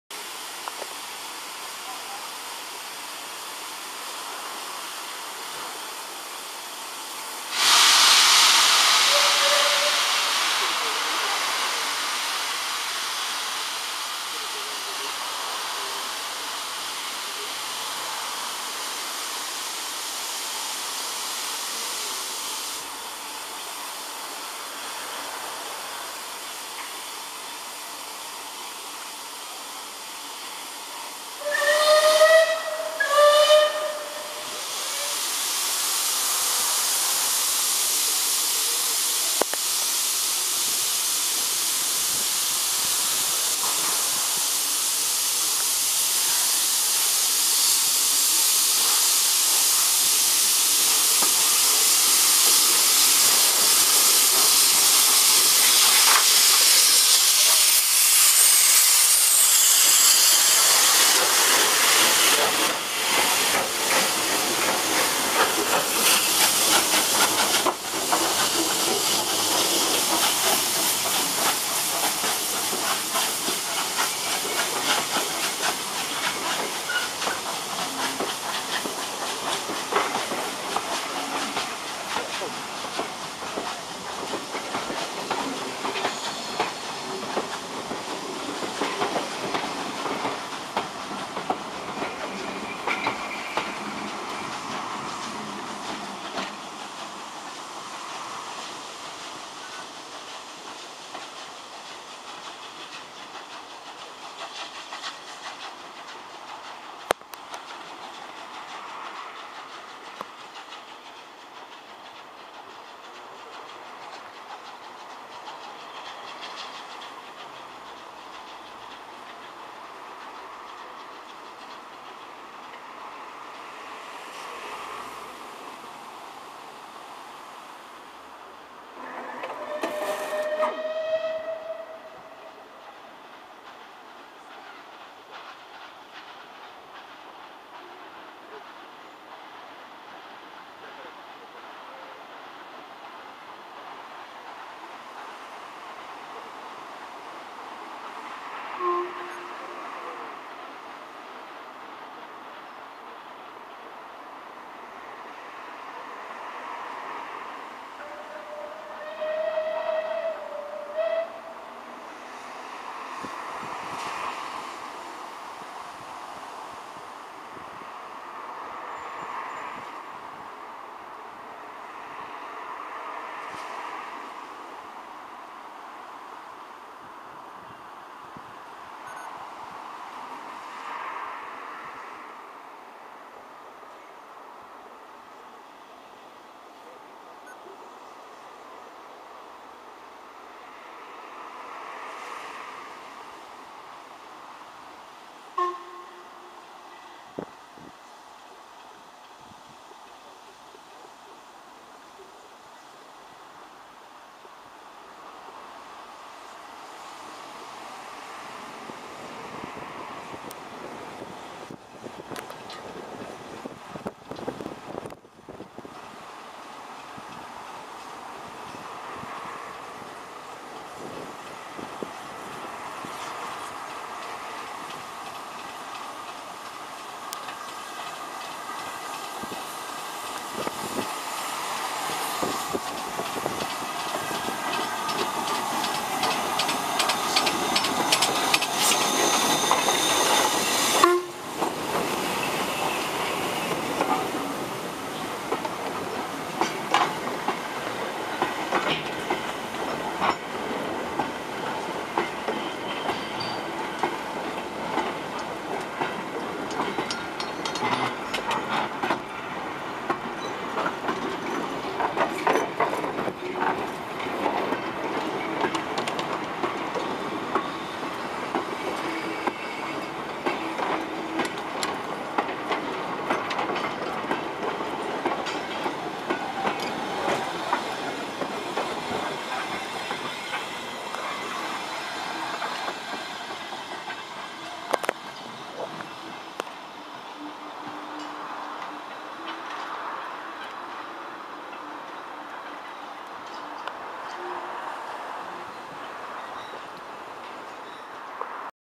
Mid Hants Steam Gala Alton Freight and Passenger combo
West Country Class 34046 Braunton. and 34007 - SR West Country Class 'Wadebridge' departs out of Alton Railway Station and straight after 45379 - LMS Black 5 pulls into the stations sidings straight after